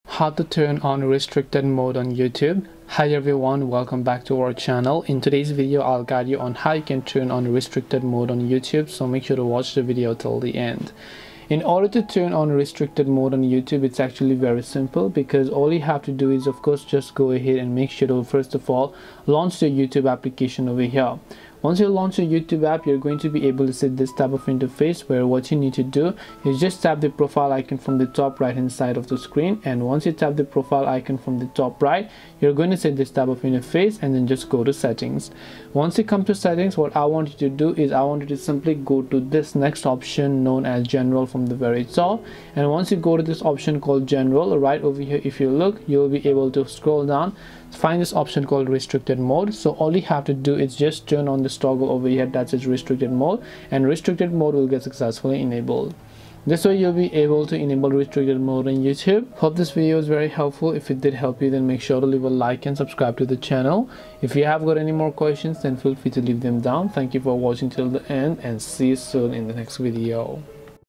How to Turn On Restricted Mode on YouTube !
This video guides you through an easy step-by-step process to enable restricted mode on YouTube. So make sure to watch this video till the end.

~ Chapters:
0:00 Introduction
0:10 Turn On Restricted Mode
0:59 Conclusion